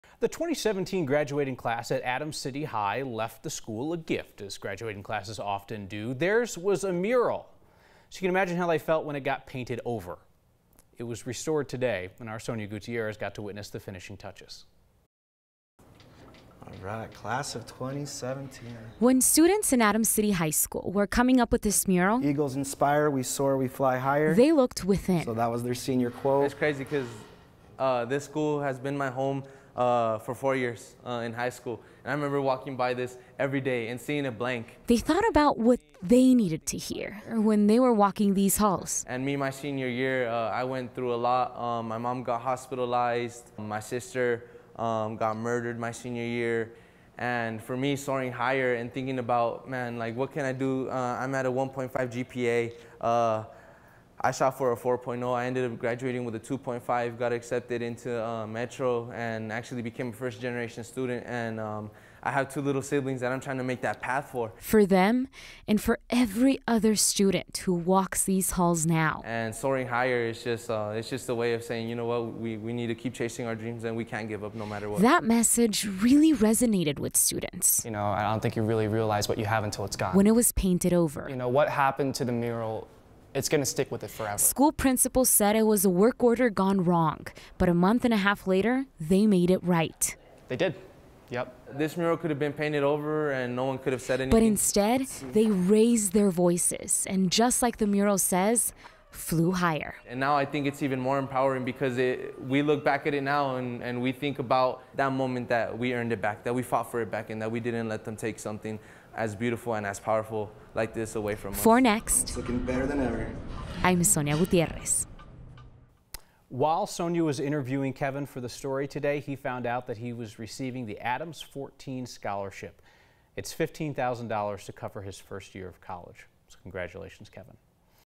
Artist returns to high school to repaint inspiring mural
The 2017 graduating class at Adams City High School left the school a gift - a mural. Then it got painted over. The school has since owned up to the mistake and brought in artist Jay Jaramillo to restore it.

9NEWS (KUSA) is located in Denver, Colorado.